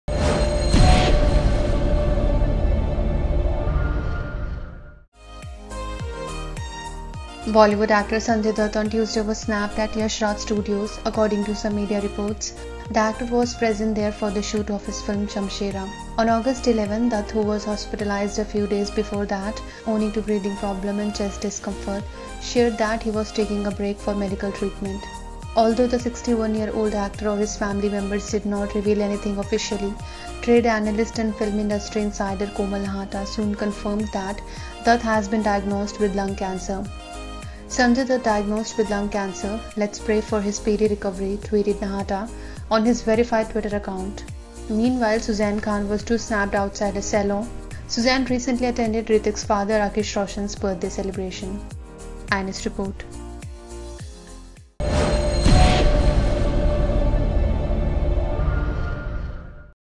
Sanjay Dutt snapped at Yash Raj studios
Bollywood actor Sanjay Dutt on Tuesday was snapped at Yash Raj studios.